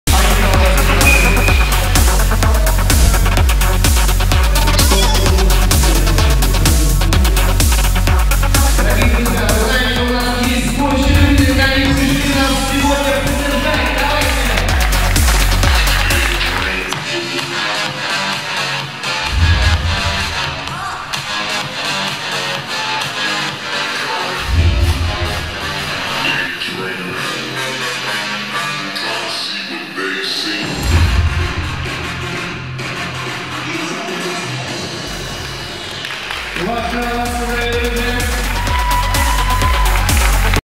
Чирлидинг СПК "ЯРОПОЛК". Турнир по самбо, 05.11.2017 г.
5 ноября 2017 г. в СПК "Ярополк" состоялся внутренний турнир по самбо среди спортсменов клуба, посвящённый празднику- "День Народного Единства".
В соревновании приняли участие 52 воспитанника СПК "Ярополк" от 10 до 30 лет.